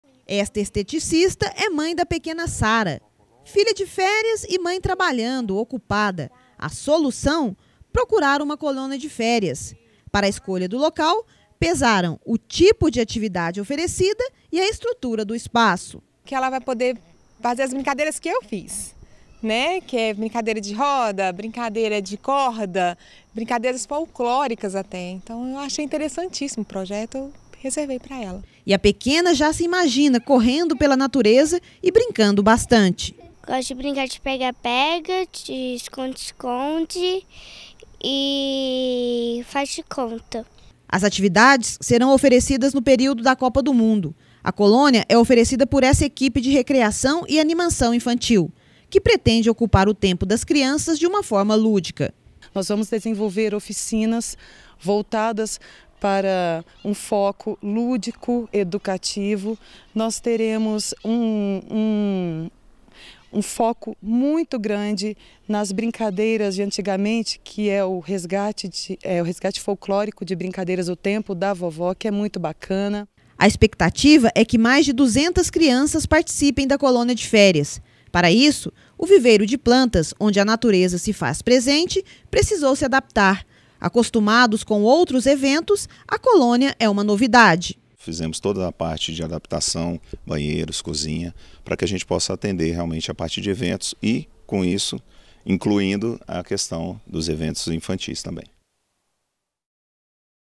Copa + Férias - Colônias de Férias são opções para período de recesso escolar
Reportagem de Gabriel Guimarães, narração de Kátia Medeiros, com imagens de Romens Almeida para o Jornal da Vitoriosa e Portal Uipi de 05/06/2014